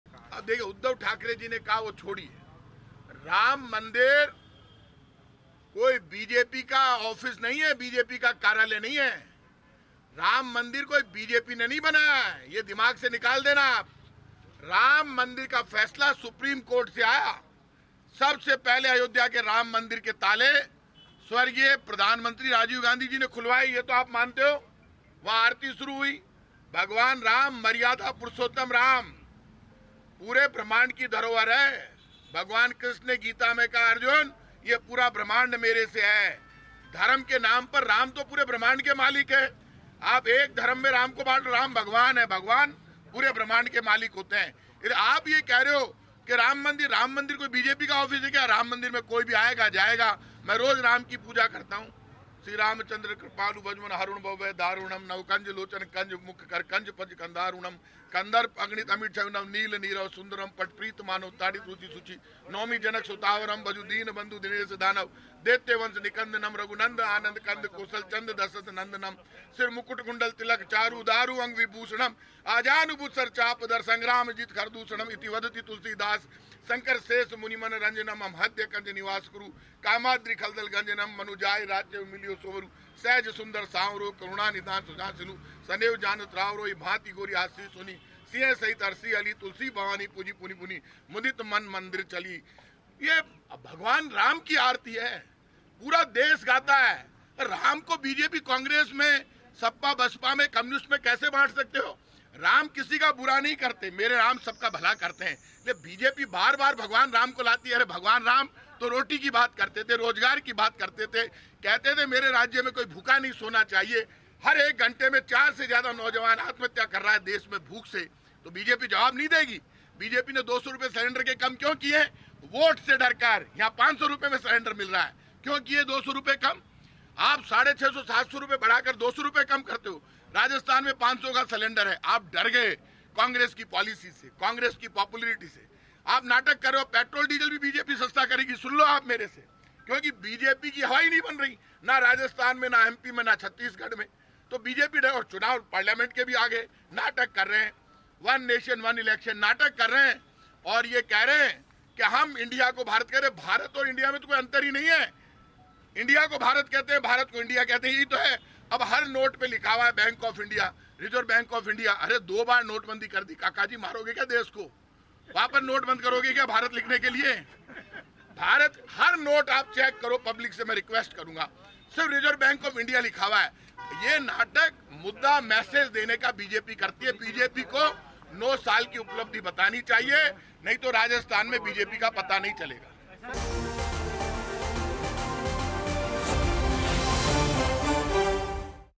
Rajasthan Election: 'राम सबके हैं तो राम मंदिर BJP का कैसे', Congress ने पूछा सवाल | Chunav Aaj Tak
राजस्थान के मंत्री प्रताप खाचरियावास ने कहा है कि राम मंदिर कोई बीजेपी का ऑफिस नहीं है। जिसकी इच्छा होगी, उस मंदिर में जाएगा। उद्धव ठाकरे के हालिया बयान के बारे में पूछे जाने पर उन्होंने कहा कि नौ साल सरकार में रहने के बाद बीजेपी के पास जनता को बताने के लिए कोई उपलब्धियां नहीं हैं। वह इसलिए बार-बार राम मंदिर की चर्चा कर रही है। उन्होंने दावा किया कि राम मंदिर बीजेपी ने नहीं बनवाया। इसका आदेश सुप्रीम कोर्ट ने दिया था। मंदिर का ताला पहली बार राजीव गांधी ने खुलवाया था।